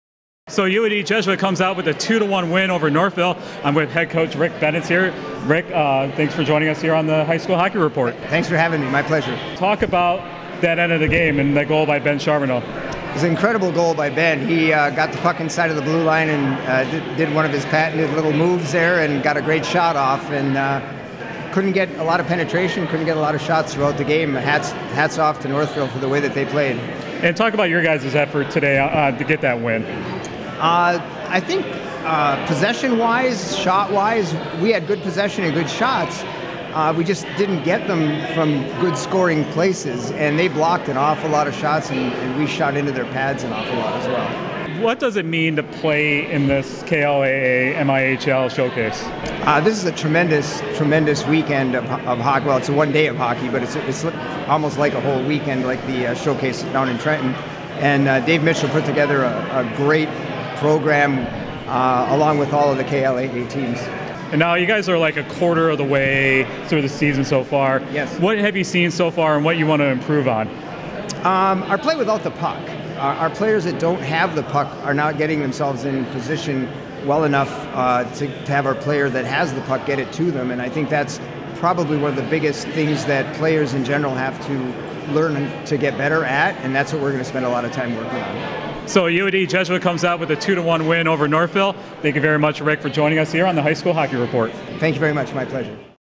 UofD Jesuit | Rick Bennetts | MIHL-KLAA Showcase Interview
Jonathon Kidd talks with UofD Jesuit head coach Rick Bennetts after the Cubs win over Northville at the MIHL-KLAA Showcase